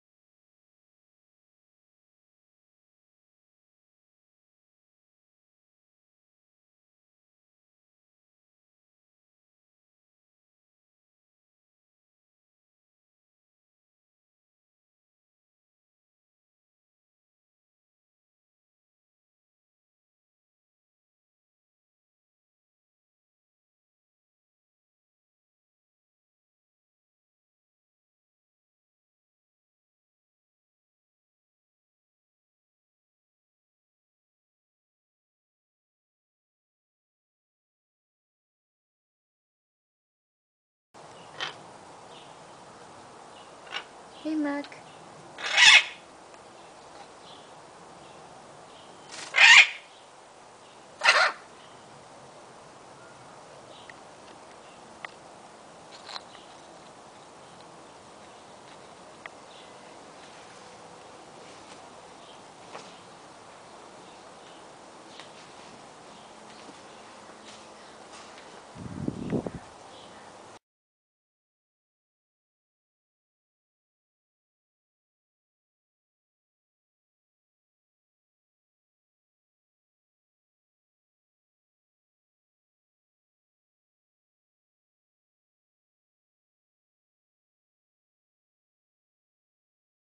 Illiger Macaw - Aviary Gym
Mac and Kusi (Illiger Macaws), This was Mac's first day here. They not only enjoy this cage alot but i get more excited screams from them both to take them out of the house and be put in here daily.